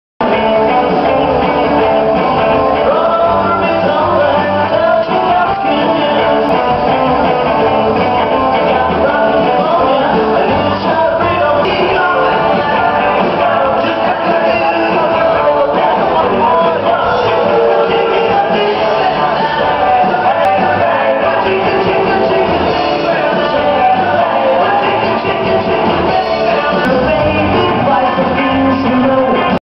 The Beatles Medellin. = JuglaresNY
grupo musical imitador de los Beatles en una  de sus presentaciones en el teatro Lido de la capital antioquena.